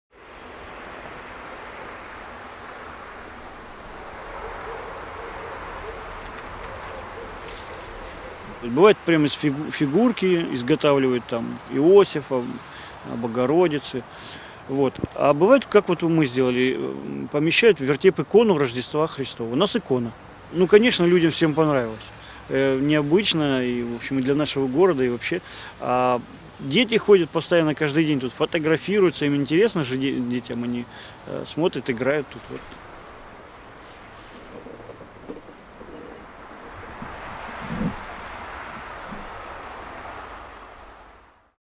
Подворье Албазинского женского монастыря украсил ледяной ...
Скульптуру сделали прихожане и послушники храма Ссылка на сайт / Видео предоставлено ТК "Альфа канал"